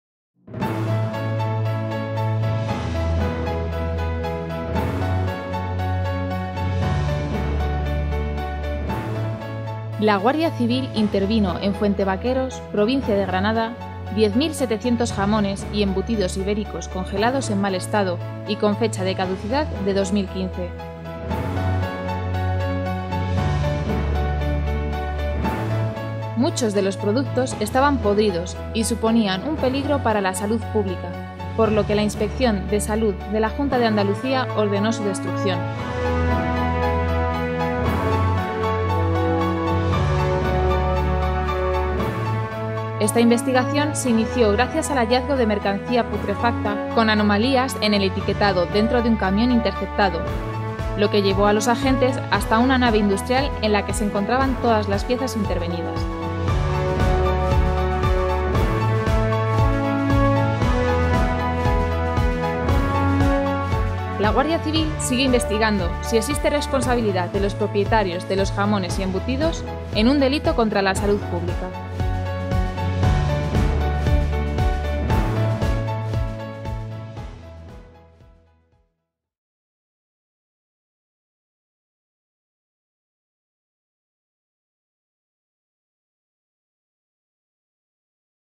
La Guardia Civil interviene 10.700 jamones y embutidos ibéricos congelados en mal estado
Los jamones y embutidos intervenidos presentaban graves anomalías en el etiquetado, estaban caducados desde 2015 y muchos de ellos mostraban también síntomas de putrefacción

La Inspección de Salud de la Junta de Andalucía ha ordenado su inmediata destrucción al suponer un peligro para la salud pública

8 de julio de 2018.- La Guardia Civil ha descubierto, en un polígono industrial de la localidad de Fuente Vaqueros (Granada), un almacén frigorífico donde se guardaban10.700 piezas entre jamones ibéricos congelados y embutidos de cerdo ibérico envasados, también congelados, en mal estado de conservación.

El pasado día 26 de junio una patrulla de la Guardia Civil de Pinos Puente interceptó, en las inmediaciones del polígono La Fuente, un camión frigorífico cargado con 25 palés de jamones ibéricos y embutidos de cerdo ibérico congelados  que presentaban anomalías en el etiquetado y algunos de los jamones también síntomas de putrefacción.

Tras constatar los agentes que el camión interceptado se dirigía hasta una nave industrial cercana, procedieron a la inspección de dicha nave que albergaba dos grandes cámaras frigoríficas, en las que se guardaban 10.700 piezas entre jamones y embutidos de cerdo ibérico congelados que presentaban las mismas anomalías que las piezas intervenidas en el camión. Muchas estaban caducadas desde el año 2015, otras ni siquiera tenían fecha de caducidad, algunas estaban visiblemente podridas, no existían albaranes que acreditaran la procedencia ni el destino del producto y algunas de las etiquetas inspeccionadas presentaban indicios de haber sido manipuladas.

La Guardia Civil inmovilizó la mercancía al considerar que pudiera suponer un riesgo para la salud pública y se alertó a la Delegación Provincial de Salud de la Junta de Andalucía.

La Inspección del Distrito Sanitario Metropolitano de Granada ha ratificado posteriormente las medidas cautelares que tomó la Guardia Civil respecto de los jamones y embutidos intervenidos, y la Inspección de Salud de la Junta de Andalucía ha ordenado su completa destrucción por suponer un riesgo para la salud pública.

La  Guardia Civil continúa investigando si existe responsabilidad de los propietarios de los jamones y de los embutidos intervenidos en un delito contra la salud pública.